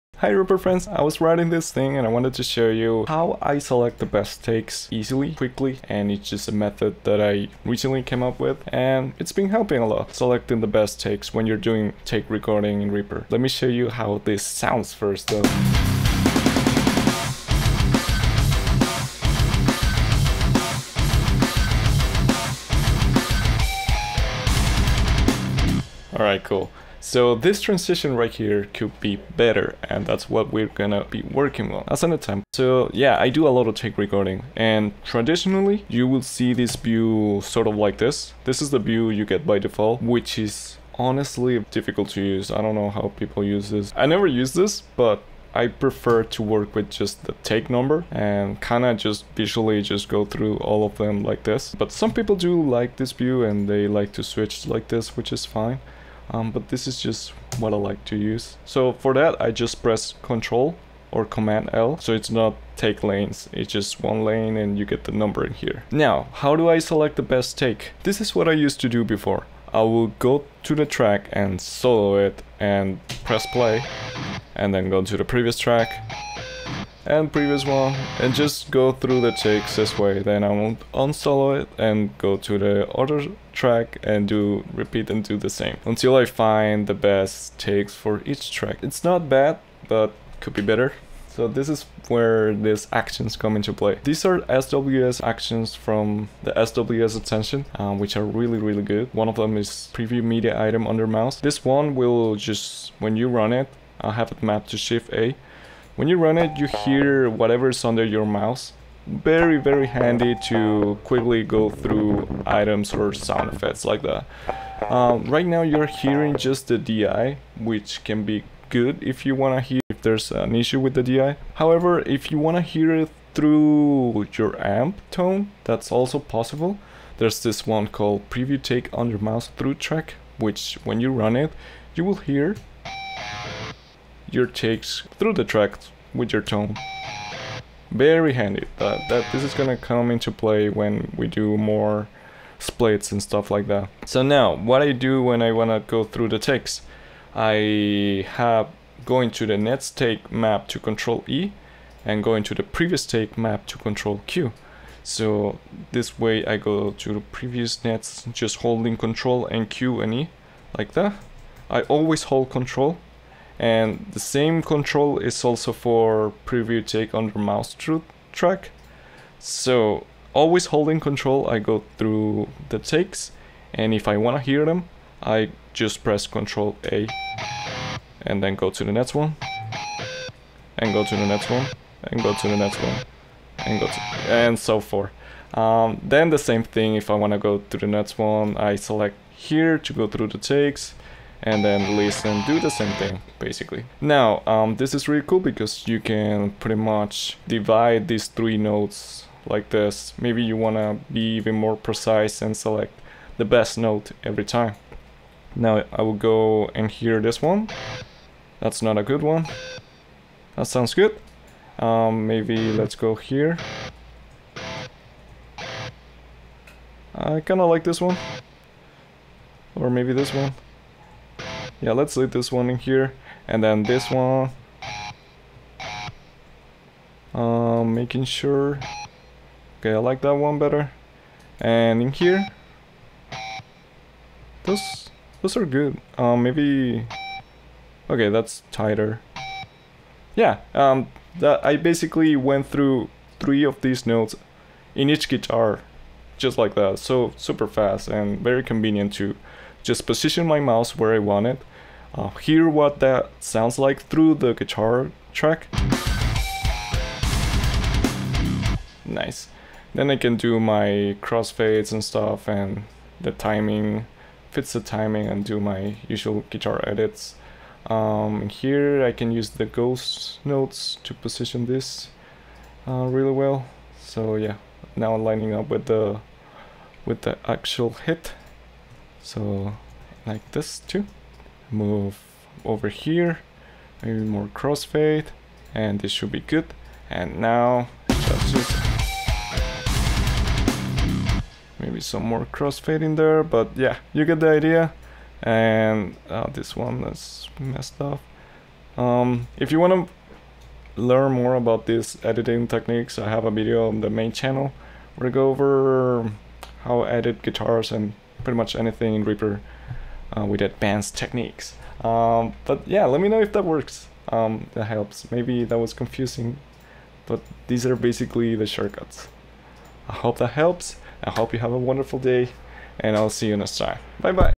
if you do take recording in REAPER...
Showing you a way to go through takes quickly. pretty helpful to select the best takes in different tracks.

Here are the actions I use:

Ctrl+Q | Take: Switch items to next take
Ctrl+E | Take: Switch items to previous take
Ctrl+A | SWS/BR: Preview take under mouse through track

Hey – In this channel I upload quick roughly edited reaper tips here that I just wanna share with reaperfriends.